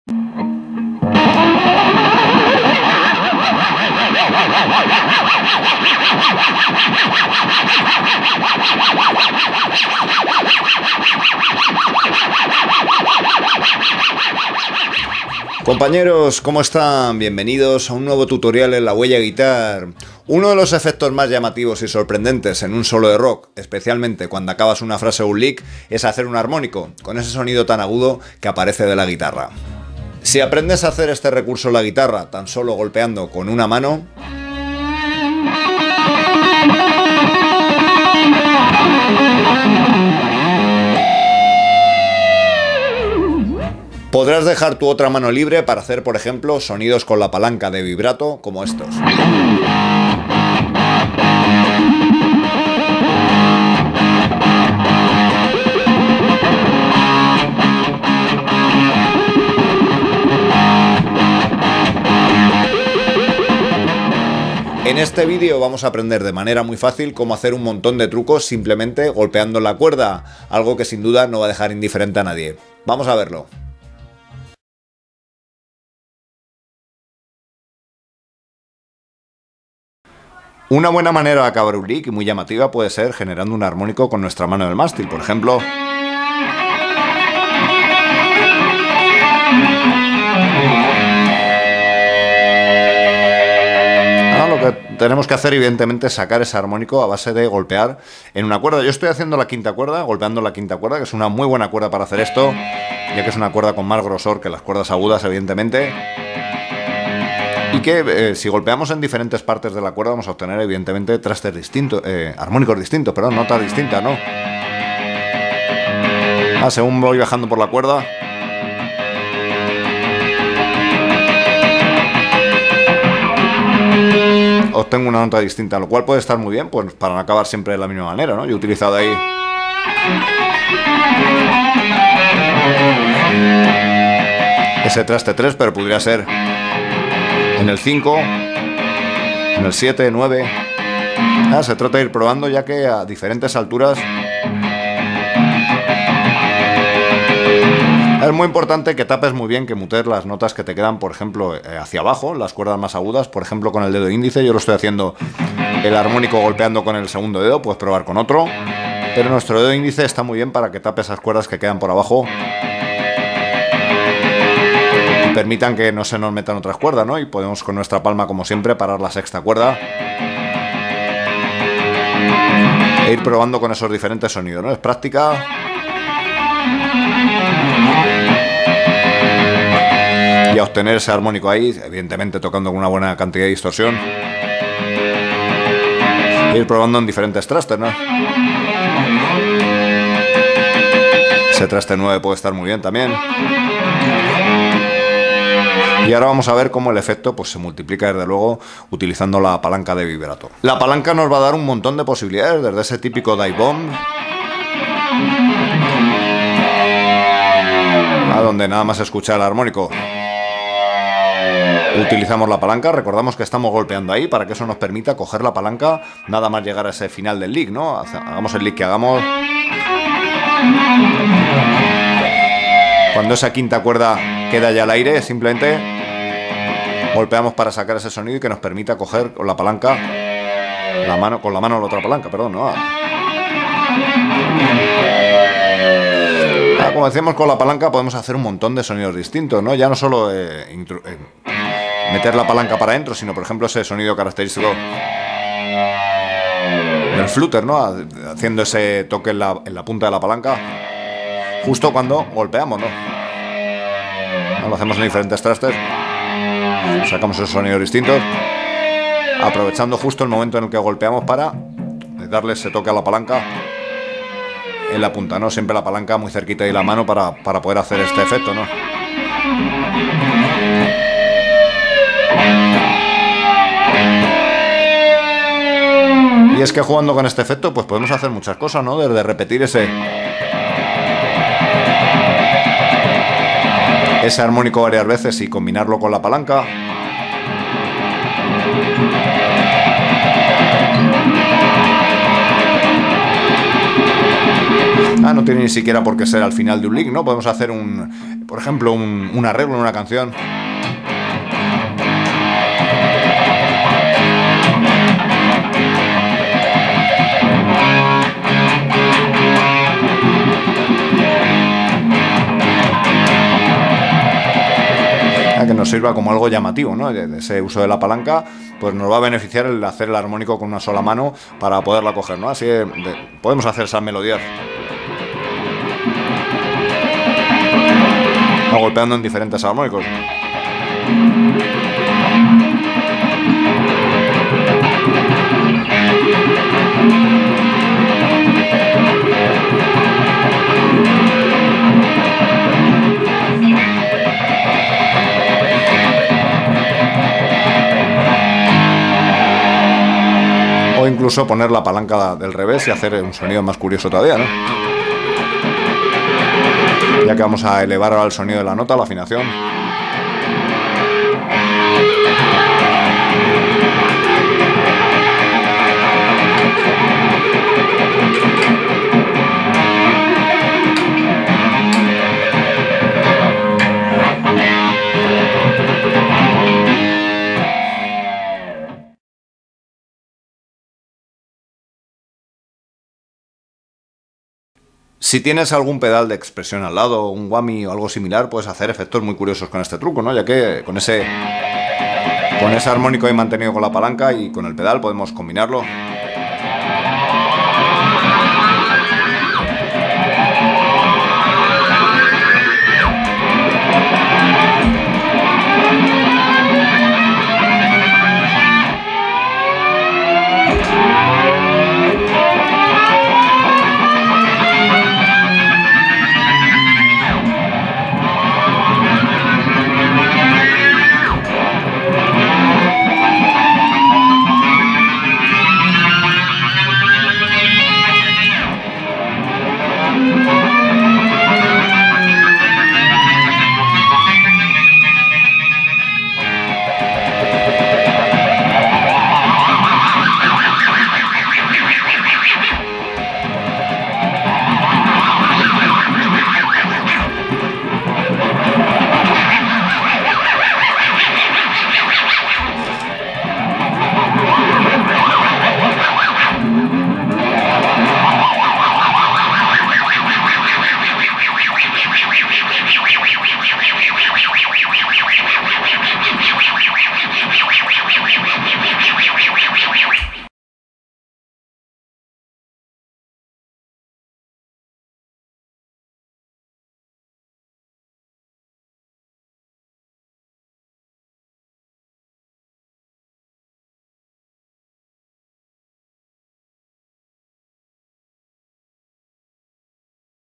POR QUÉ GOLPEANDO CON SÓLO UNA MANO LA CUERDA...PODRÁS SACAR ESTOS 10 SONIDOS ESPECTACULARES!!
En "POR QUÉ GOLPEANDO CON SÓLO UNA MANO LA CUERDA...PODRÁS SACAR ESTOS 10 SONIDOS ESPECTACULARES" saca sonidos muy llamativos en guitarra eléctrica  tocando sólo una nota a través de armónicos combinados con otros recursos!!A estilo de guitarristas como DIMEBAG DARREL,REB BEACH,VAN HALEN,STEVE VAI etc..combínalo con efectos como la palanca de vibrato/whammy bar en tus solos y licks de rock/metal,y crea efectos inreibles con ello!!En este video tutorial,"10 TRUCOS GOLPEANDO CON SÓLO UNA MANO LA CUERDA...QUE TE DARÁN UNA TÉCNICA ESPECTACULAR",aprende a tocar en guitarra eléctrica una de las técnicas más curiosas junto a otras como el tapping,barridos,rápido legato etc.. e incluso otro tipo de armónicos (tap,de arpa,artificiales etc...)
On "10 TRICKS JUST HITTING THE STRING WITH JUST ONE HAND",learn how to play on electric guitar a natural harmonic combines with tricks such as whammy bar or expression pedals!!!